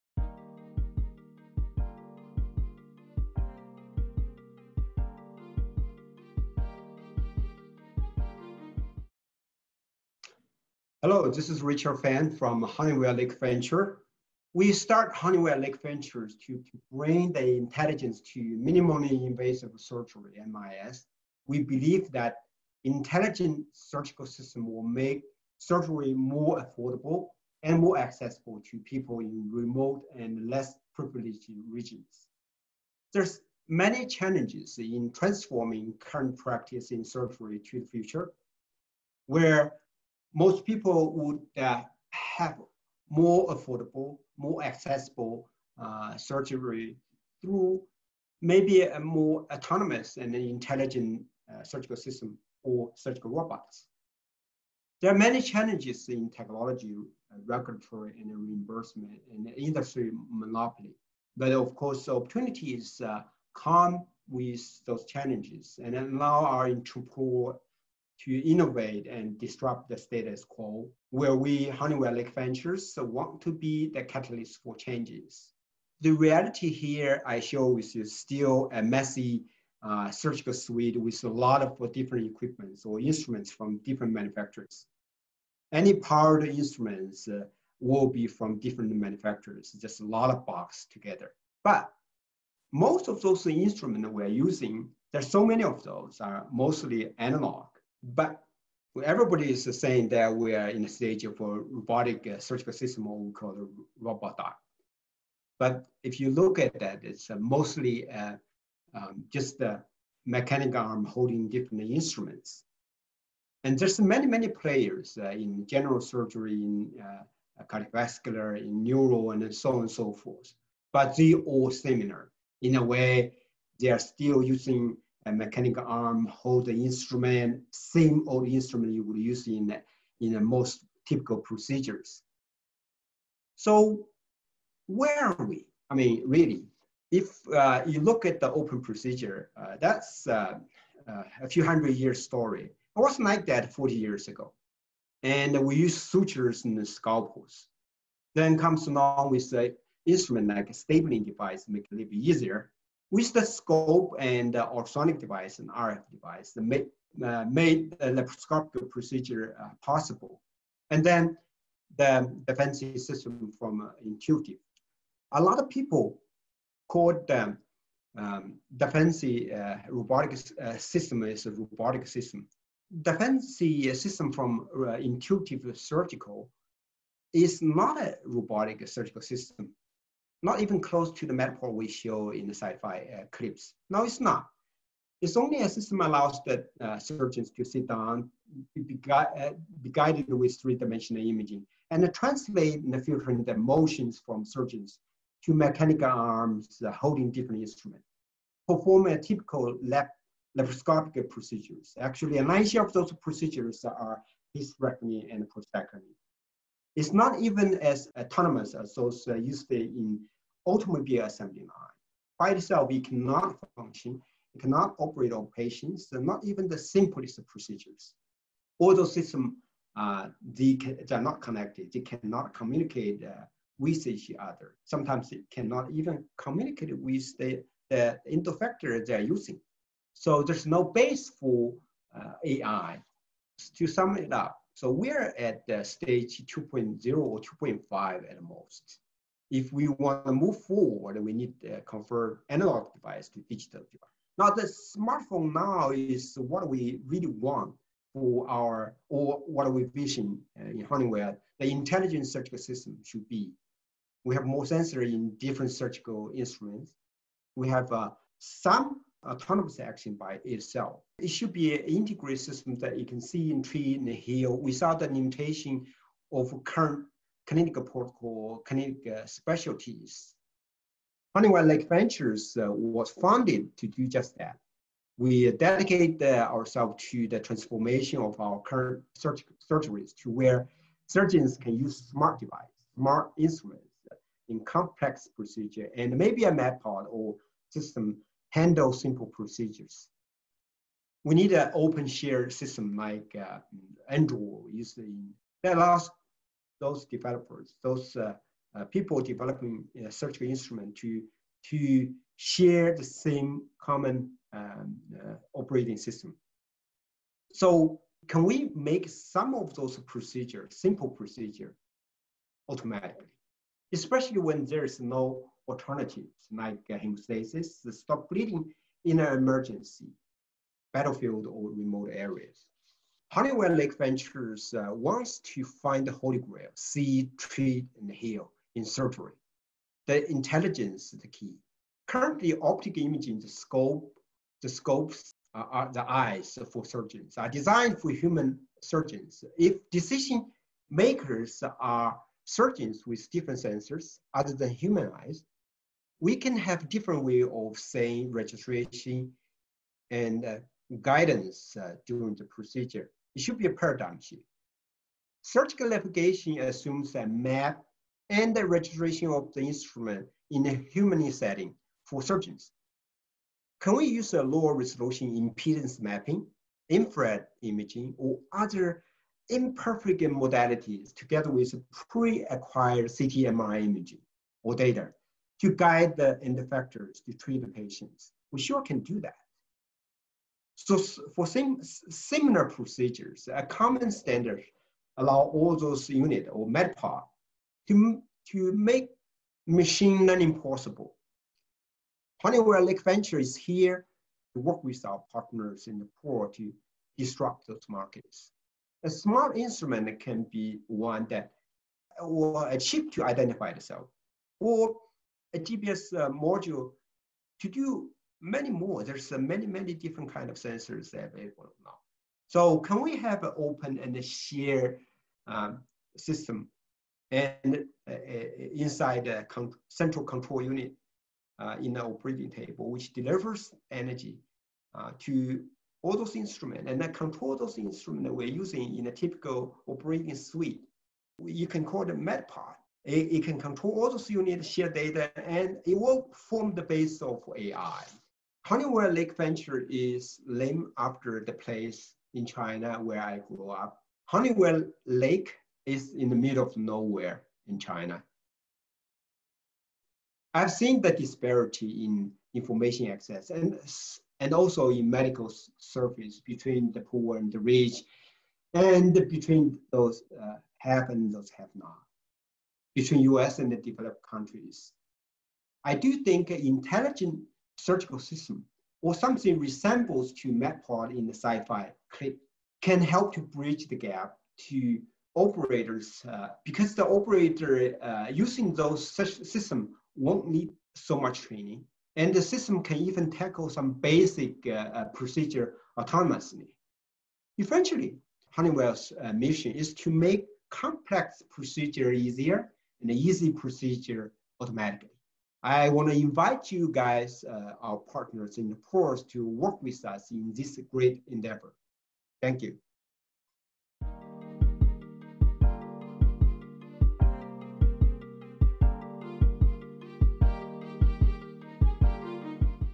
The Future of Surgery - Reality Check
In episode 5 of the F50 Summit series, Founding Partner Richard Fang leads conversations on the future of the surgical landscape and exciting upcoming technology.